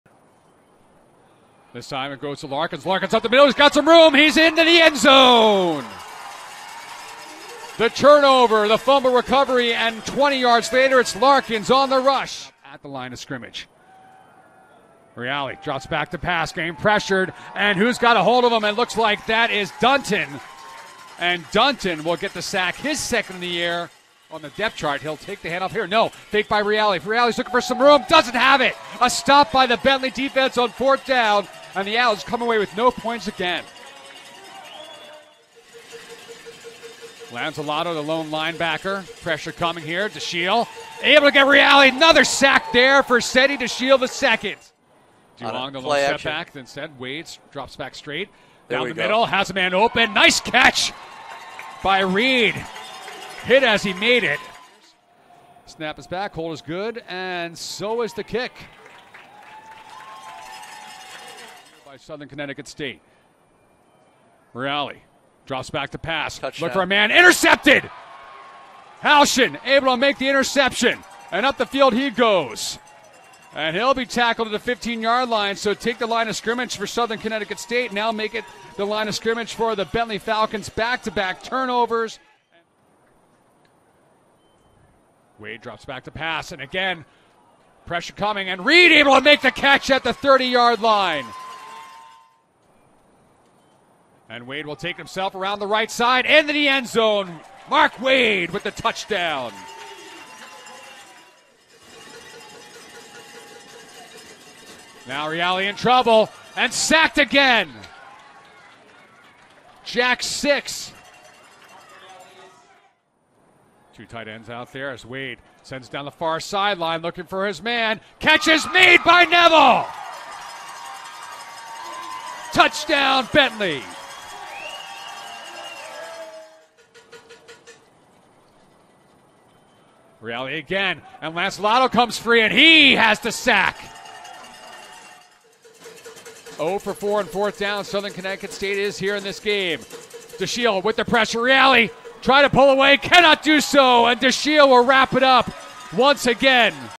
Football Highlights vs. Southern Connecticut State, Oct. 22, 2022
It was a team effort Saturday as Bentley celebrated Homecoming 2022 with its fourth straight win, 24-3 over Southern Connecticut State. The defense came up with eight sacks in another outstanding performance and 3 different Falcons scored touchdowns.